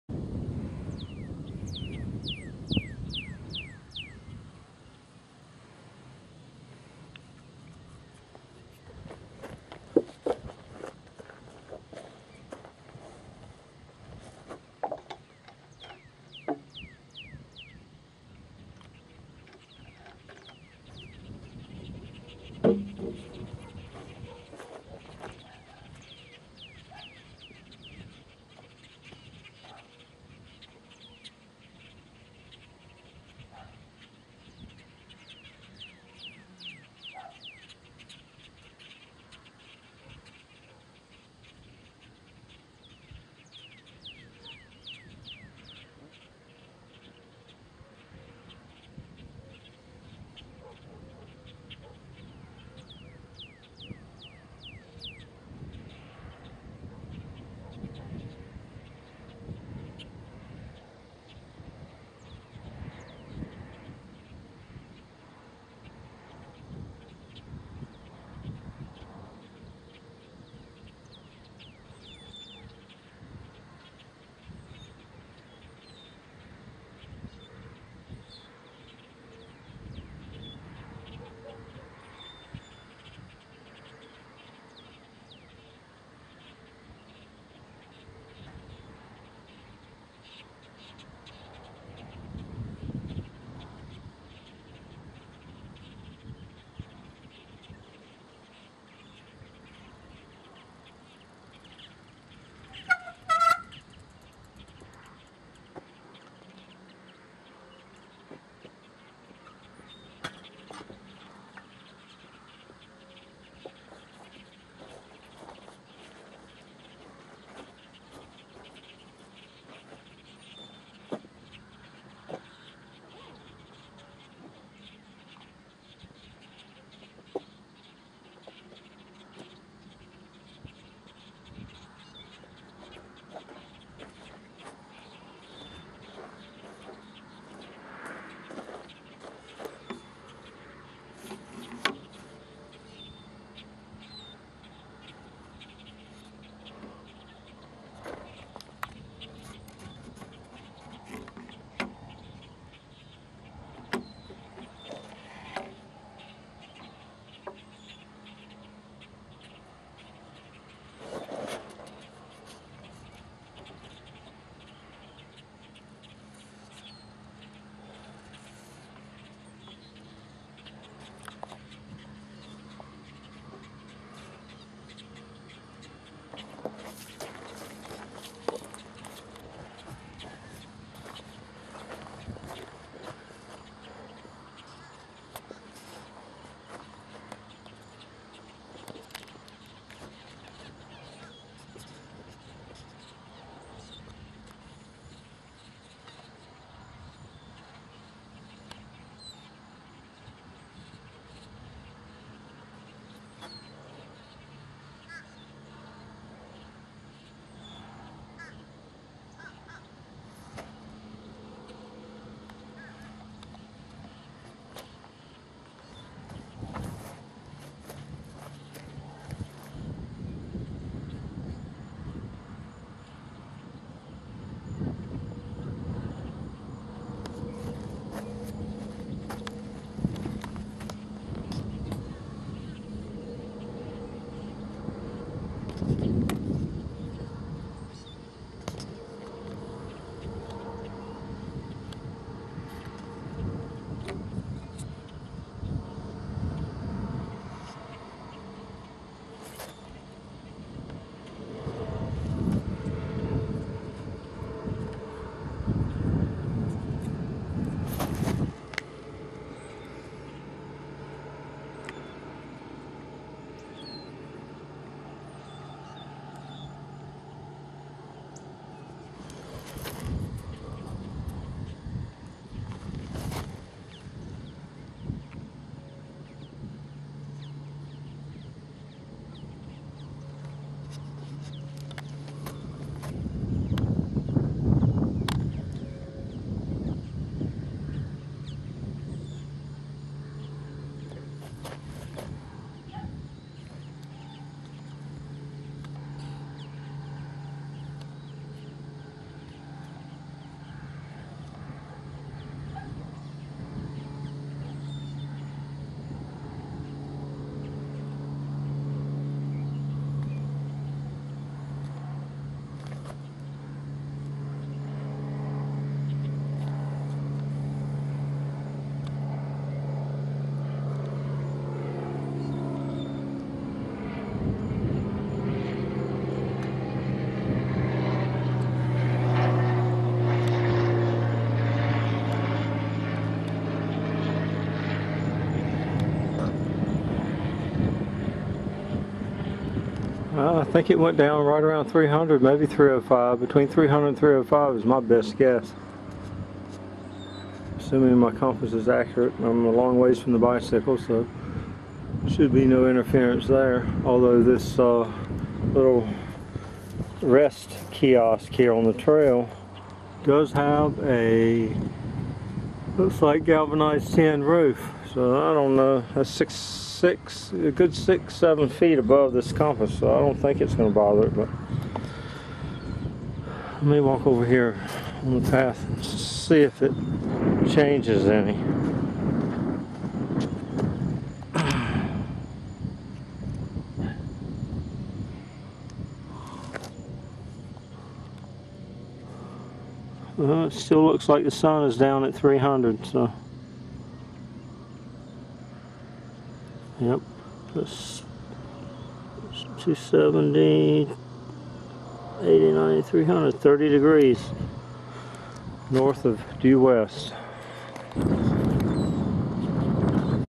okeechobee sunset June third 2011 may 2012 update
position of sunset, approx 30 to 35 degrees north of due west. or 300 to 305 degrees on the compass. looking back on this video now, it confirms that what is normal today (302.7 degrees sunset over okeechobee) was also occuring this same week in 2011.